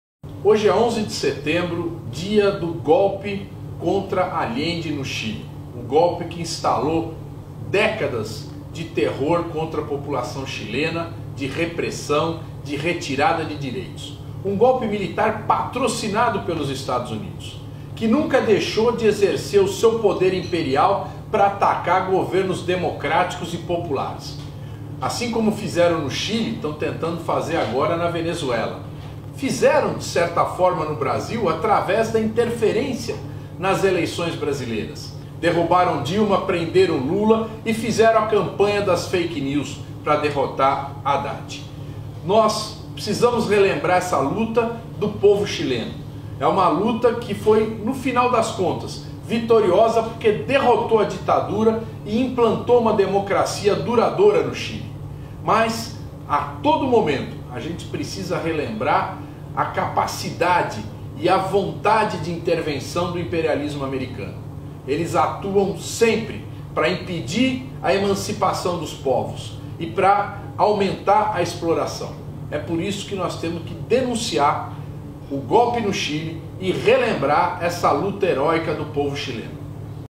11 DE SETEMBRO: 46 ANOS DO GOLPE DE PINOCHET
EUA patrocinaram o golpe militar no Chile e agora ameaçam intervir na Venezuela. Sempre usaram seu poder imperial para atacar governos democráticos e populares. No Brasil, os americanos interferiram nas eleições, apoiaram o golpe contra Dilma e a prisão de Lula. A enxurrada de fake news nas eleições para derrotar Haddad é outro exemplo de interferência dos EUA. A luta do povo chileno tem muito a nos ensinar e motivar a seguir lutando contra esse governo antidemocrático e autoritário de Bolsonaro.